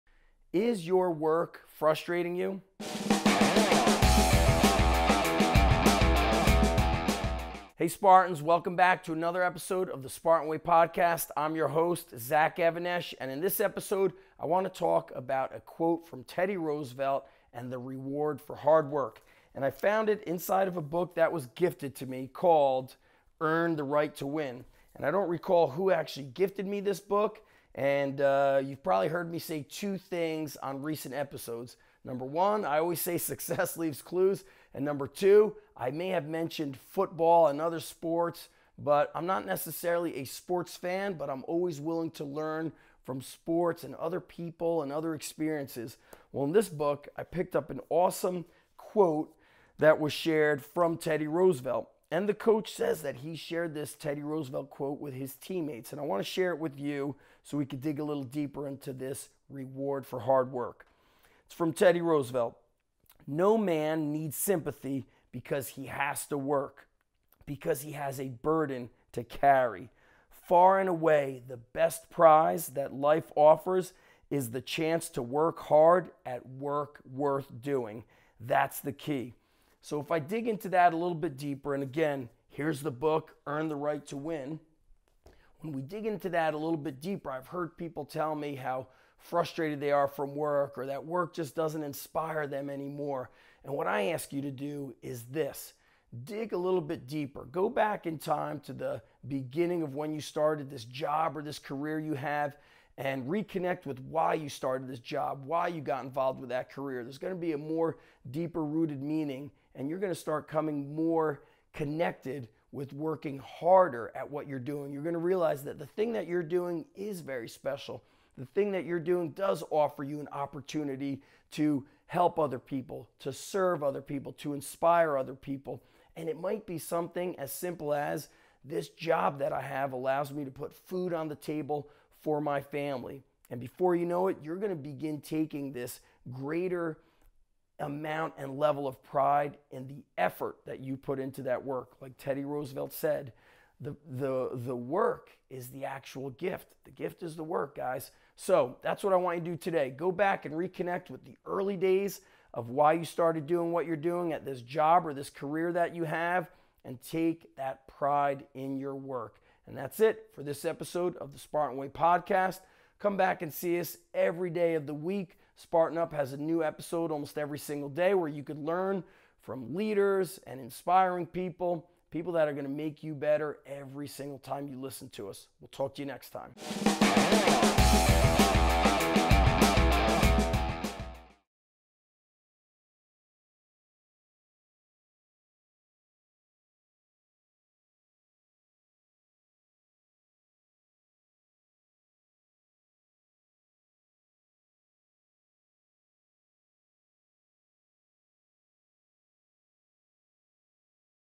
Work is the Reward // SPARTAN WAY 035
Bringing you some fire from a Teddy Roosevelt book in today’s episode! This book is a reminder that our work is a gift. It is something we GET to do, not something we HAVE to do.

LESSONS:

Reconnect with WHY you started working the job you currently have.
Find the deeper meaning behind your work.
The deeper meaning might be as simple as your job giving you the ability to put food on the table.
Take pride in your work!

CREDITS:

Producer – Marion Abrams, Madmotion, llc.
Host & Show Notes:  Zach Even - Esh